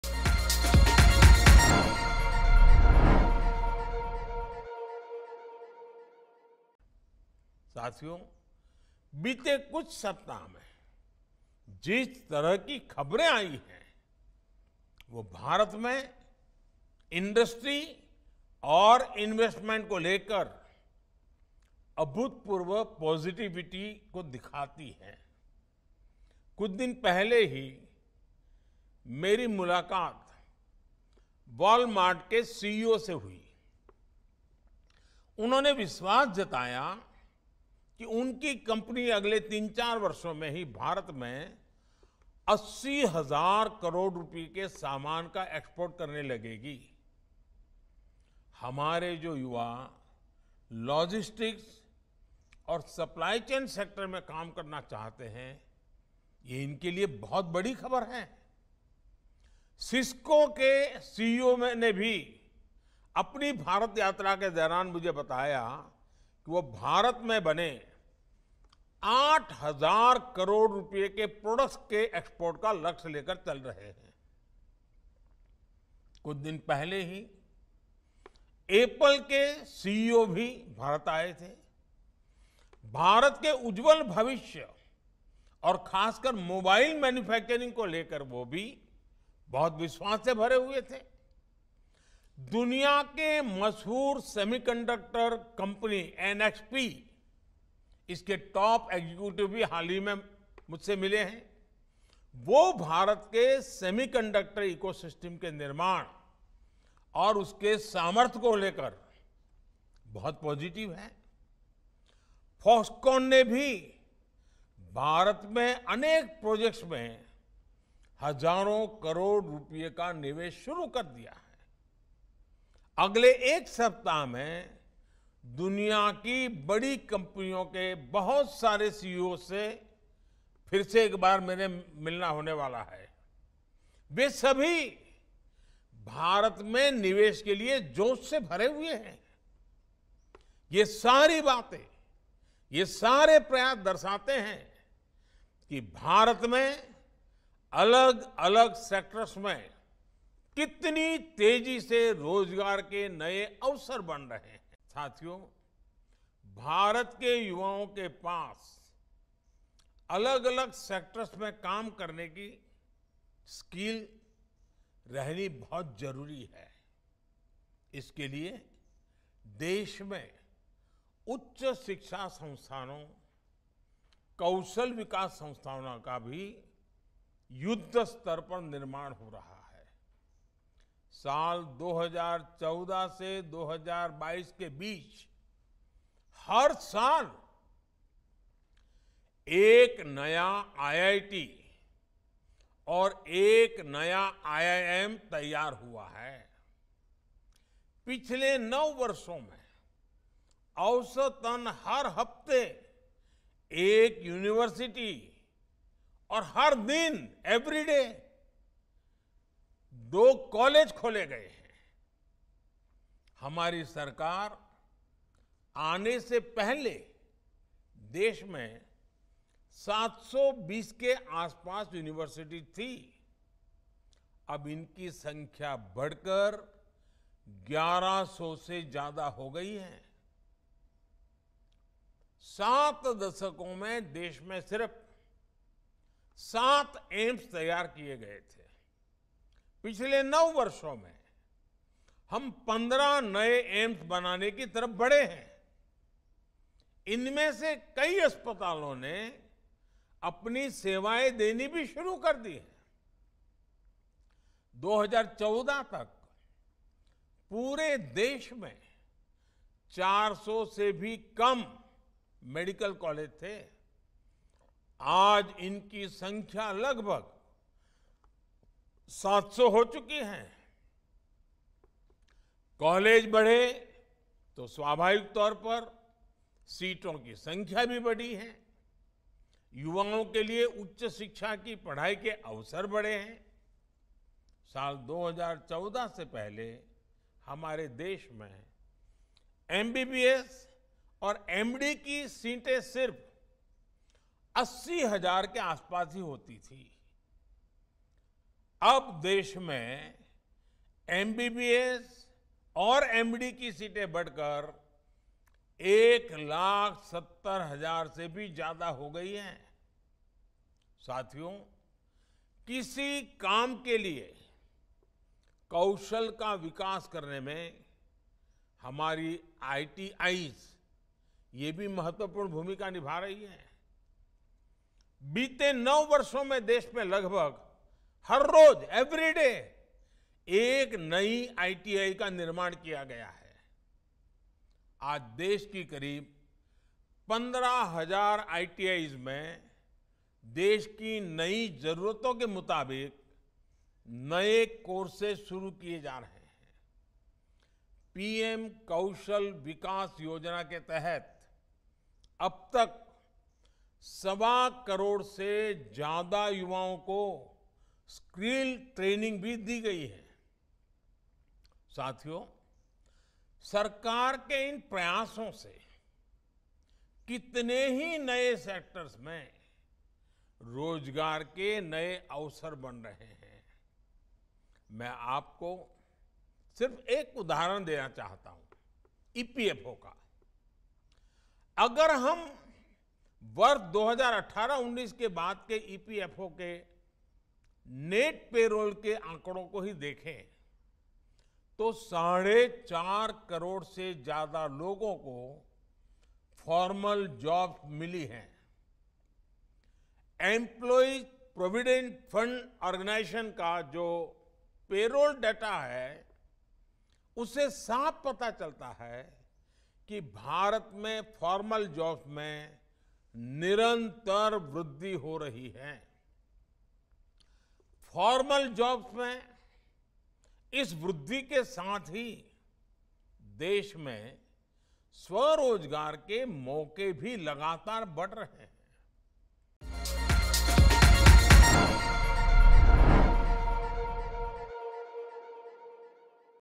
Apple, Walmart, CISCO जैसी कंपनियां भारत के सामने नतमस्तक, जानिए PM Modi क्या बोले ?
rohit pandey
pintu

thumb- भारत के सामने नतमस्तक दुनिया